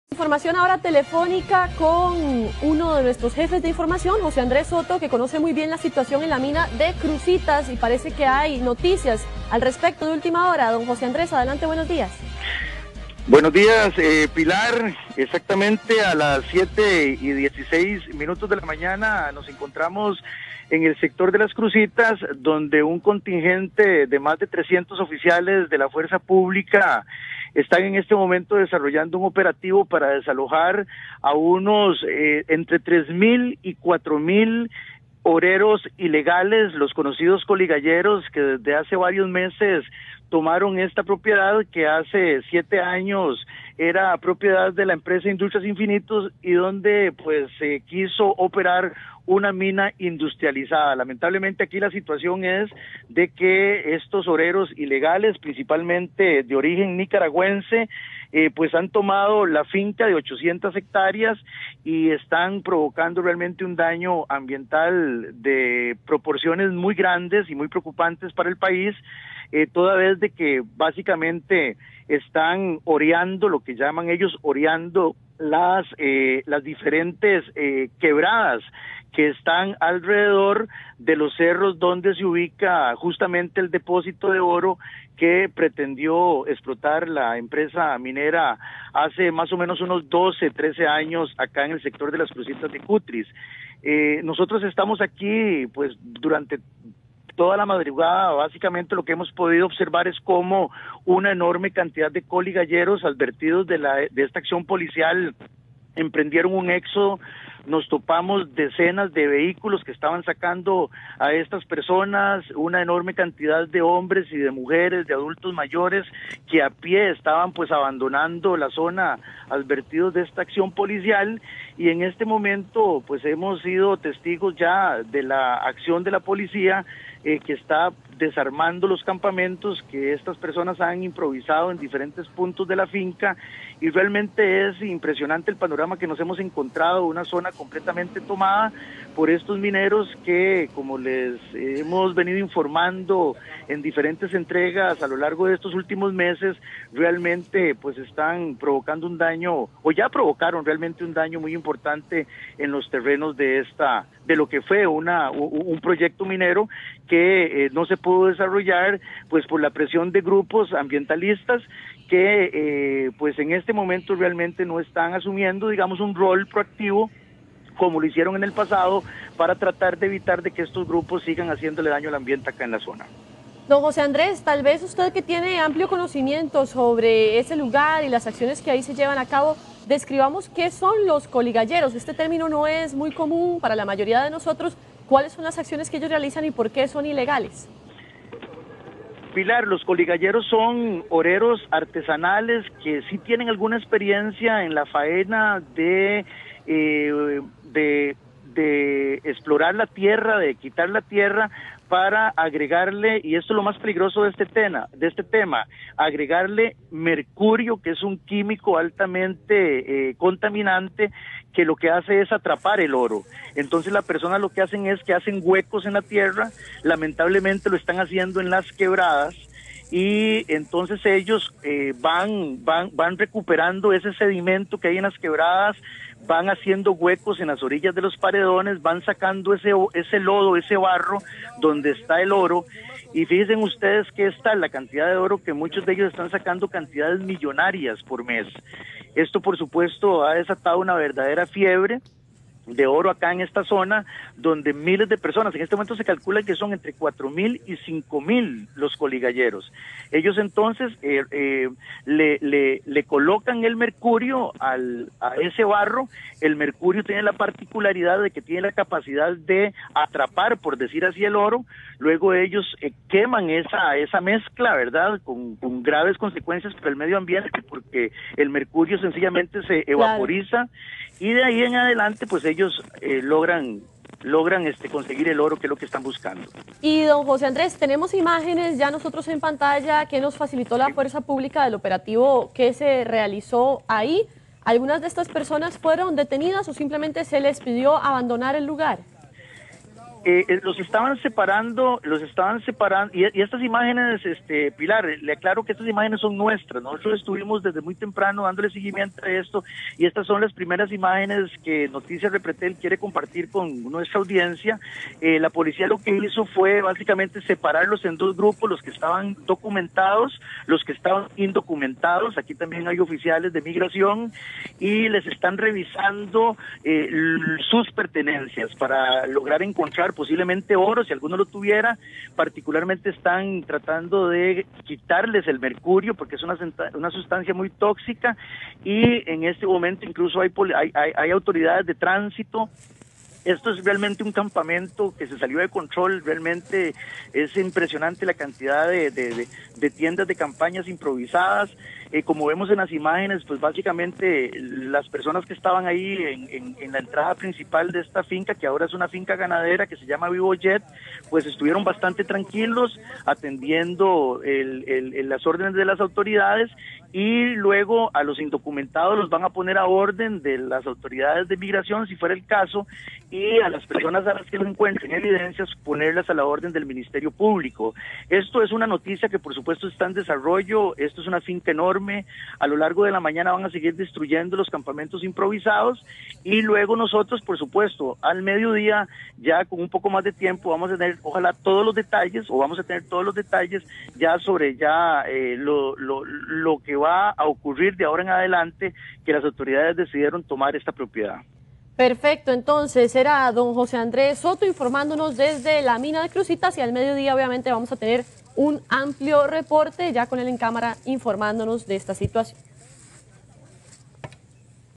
Desalojo en mina Las Crucitas
Cientos de agentes de la Guardia Rural de Costa Rica comenzaron esta mañana el desalojo de varios miles de güiriseros costarricenses y nicaragüenses de la mina Las Crucitas, cerca de la frontera con Nicaragua.